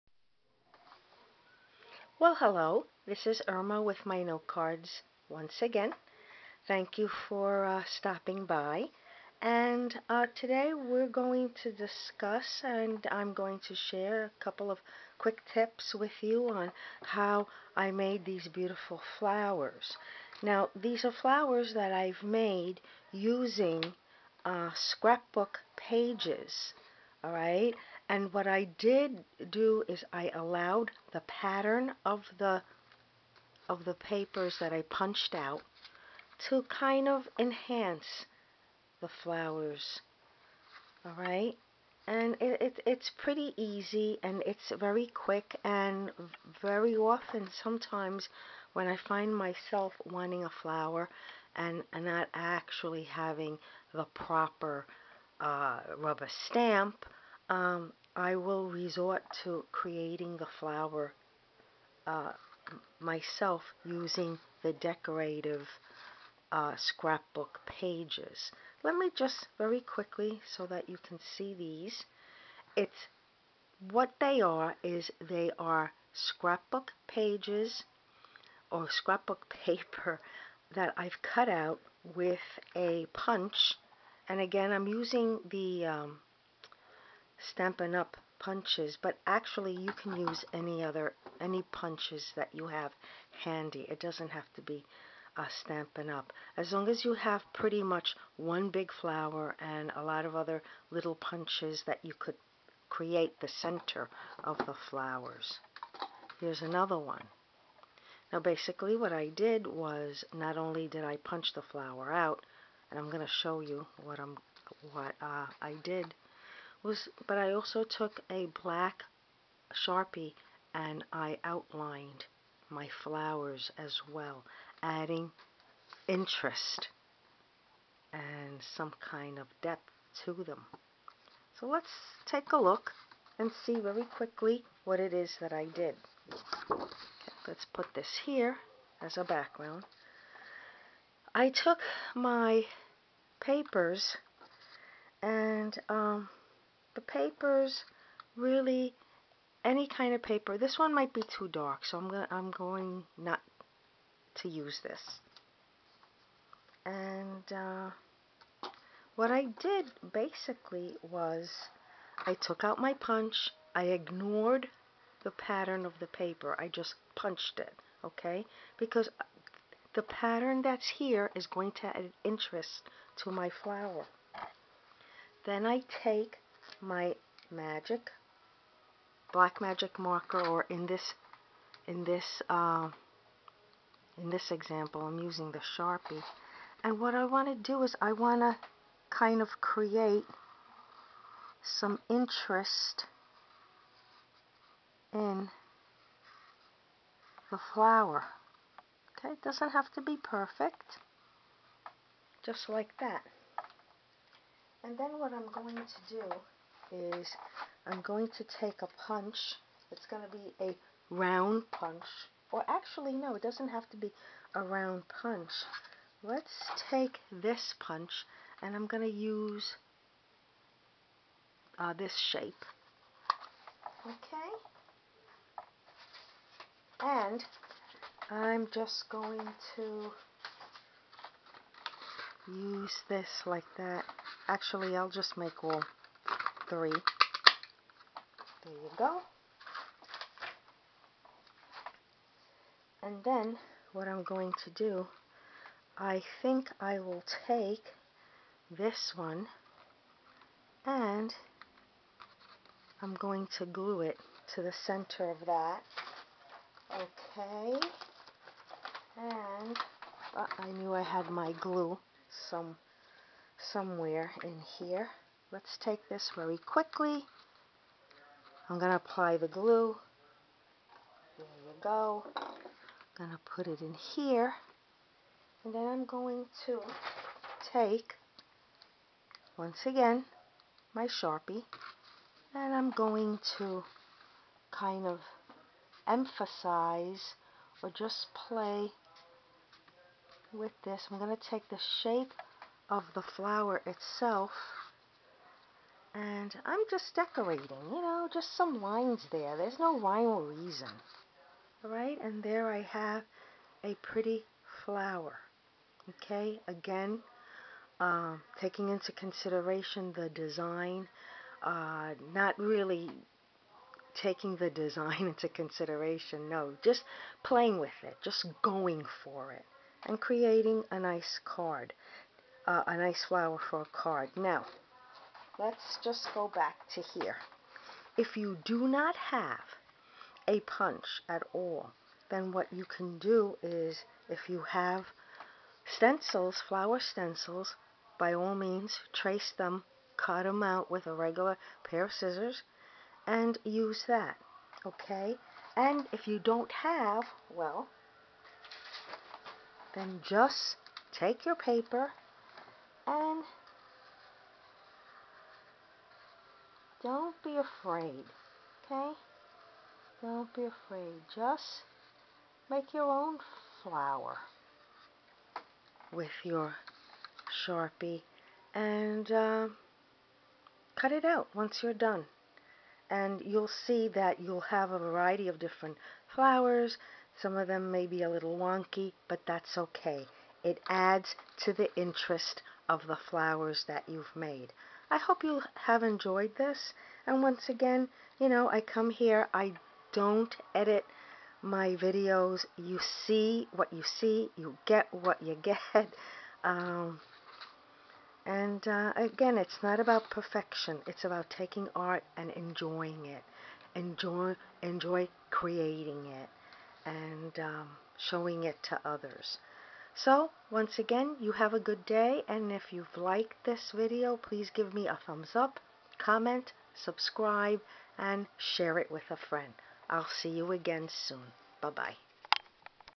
Making Flowers with Punches and Scrapbook Paper
You need not be an artist to create a pretty card with flowers in this video I show you how. Quick and easy! So take out your favorite scrapbook patterned  paper and a  couple of flower punches.